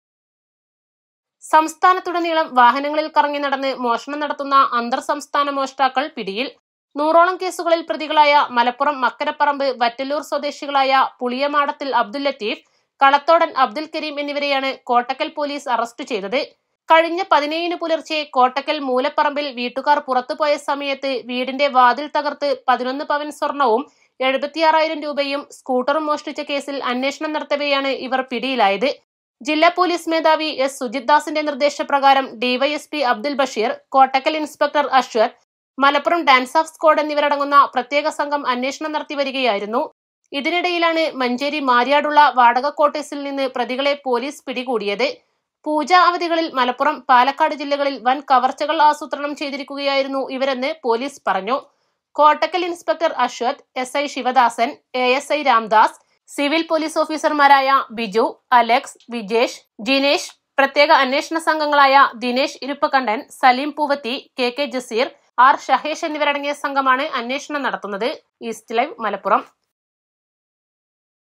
സംസ്ഥാനത്തുടനീളം കവർച്ച; നൂറോളം കേസുകളിൽ പ്രതികളായ രണ്ടംഗ സംഘം അറസ്റ്റിൽ | Malappuram
robbery across the state; In a group of two accused in around 100 cases